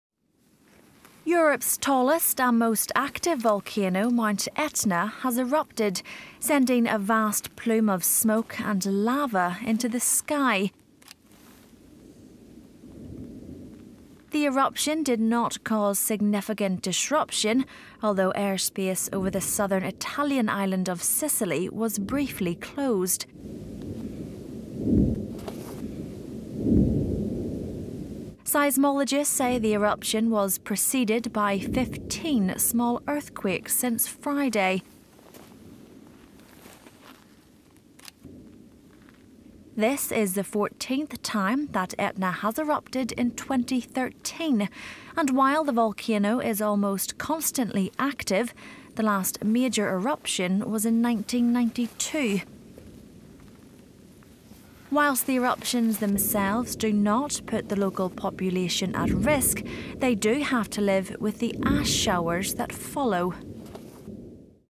Italy's Mount Etna volcano erupts sending lava and ash into sky
Europe's tallest and most active volcano Mount Etna has erupted, sending a vast plume of smoke and lava into the sky. The eruption did not cause significant disruption, though airspace over the southern Italian island of Sicily was briefly closed. Seismologists say the eruption was preceded by 15 small earthquakes since Friday. This is the 14th time that Etna has erupted in 2013 and while the volcano is almost constantly active, the last major eruption was in 1992. Report by Sarah Johnston.